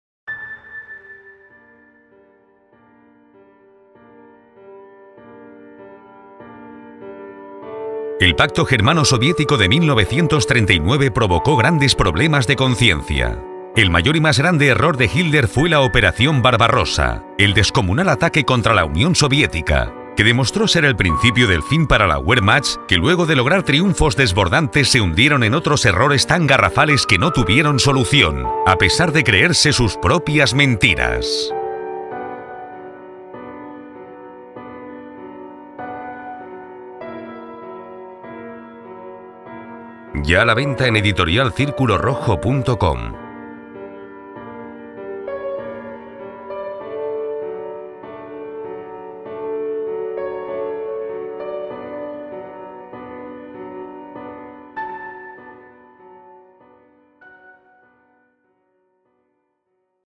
La verdad era otra (Booktrailer) - Editorial Círculo Rojo
Autor: Luis Amado.
Los hechos comenzaron en la disputa entre Alemania y Polonia por el corredor polaco de Danzing. La realidad era que Polonia era un "invento" de país hecho por los artífices del tratado de Versalles que redibujaron el mapa de Europa ignorando los perjuicios que creaban dado que Polonia había perdido su soberanía en el año 1795 al ser absorbida por Rusia, Austria y Prusia. Esa nueva Polonia fue creada a costa de territorios quitados a Alemania.
Así comenzó la guerra que terminaría siendo mundial para defender a Polonia.
Esa Polonia que se creyó todo lo que le decían y habían tramado, los ingleses y que "dejaron desamparada" tanto tos ingleses como los franceses, quienes tras prometerles toda clase de ayudas, la dejaron trágicamente sola.
Lo verdaderamente extraño fue comprobar como la URSS --en virtud del pacto con Alemania- invadió la mitad de Polonia y nadie se dio por enterado, al igual que las invasiones contra Estonia, Lituania y Letonia.
Si, en cambio consideraban un peligro mundial a Alemania cuyo propósito era el de recuperar los territorios que le fueron arrebatados en el tratado de Versalles.